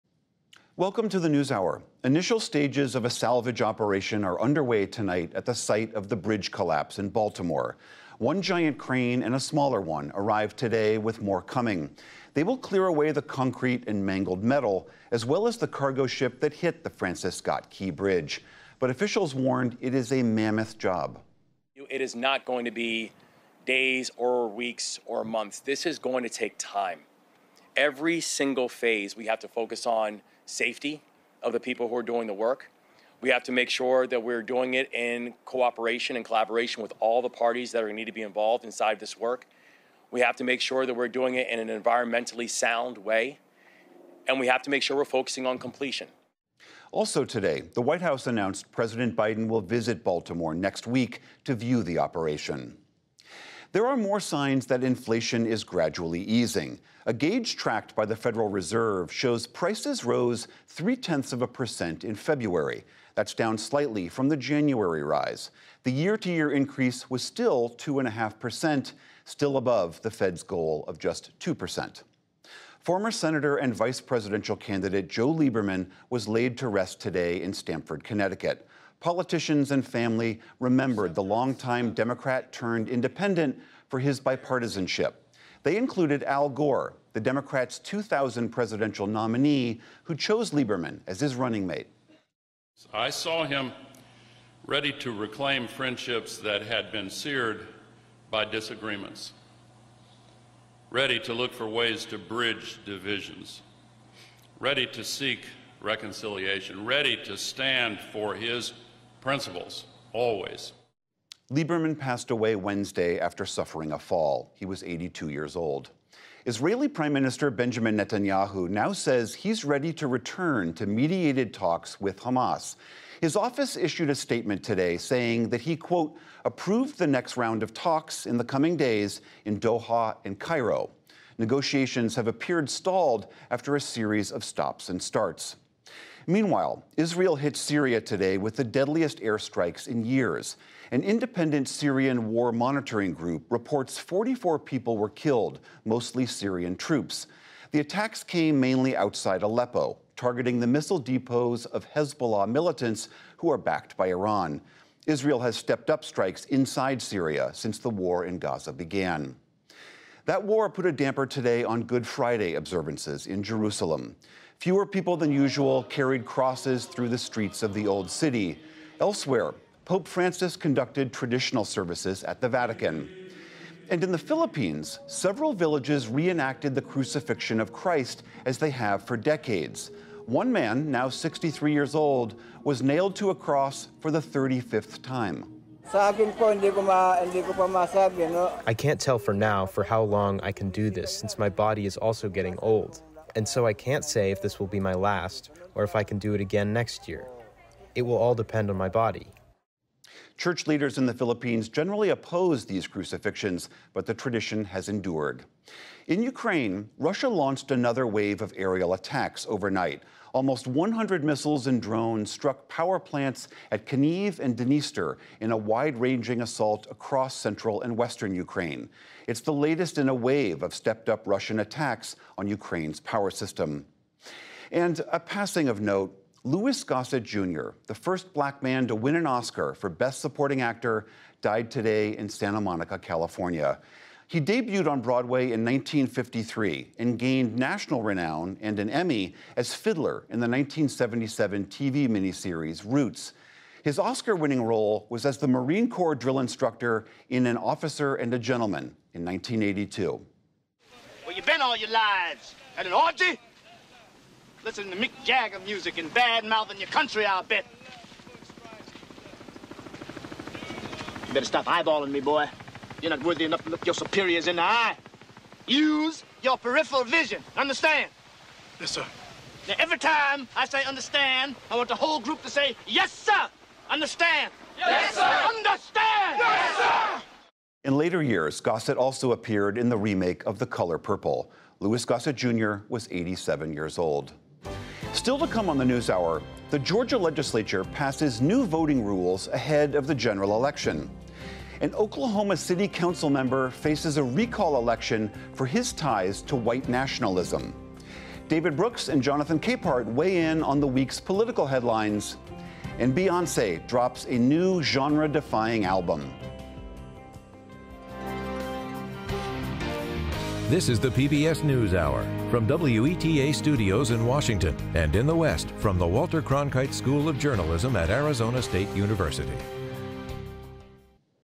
News Wrap: Salvage operation underway at the site of Baltimore bridge collapse
In our news wrap Friday, initial stages of a salvage operation are underway at the site of the bridge collapse in Baltimore, a gauge tracked by the Federal Reserve shows signs inflation is gradually easing, former senator and vice presidential candidate Joe Lieberman was laid to rest in Connecticut and Israeli Prime Minister Netanyahu says he's ready to return to mediated talks with Hamas.